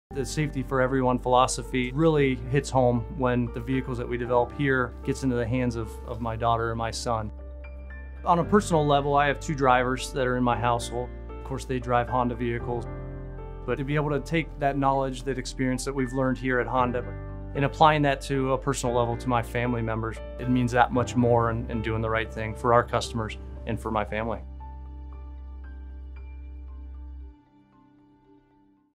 Each Honda is engineered with Safety for Everyone in mind | Ike Honda
From our own family members to yours, safety is a top priority when engineering our vehicles. When you or your loved ones get behind the wheel of a Honda, you’re driving a vehicle that’s been designed with Safety for Everyone in mind.